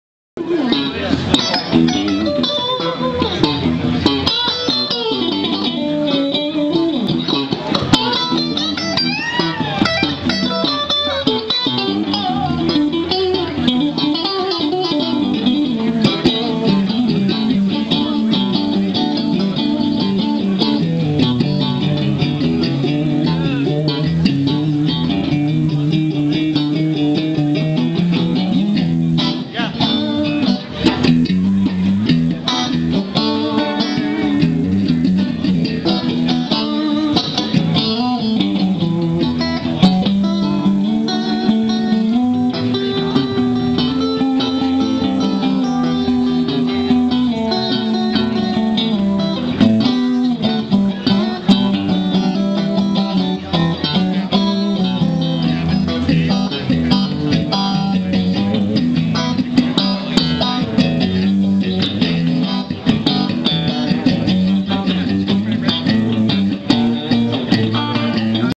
Ted "Teddy" Spanke Jammin with Uli Jon Roth
Teddy Spanke at NAMM 2010 at Mothership Booth Jammin with Mothership Edorsee ULI JON ROTH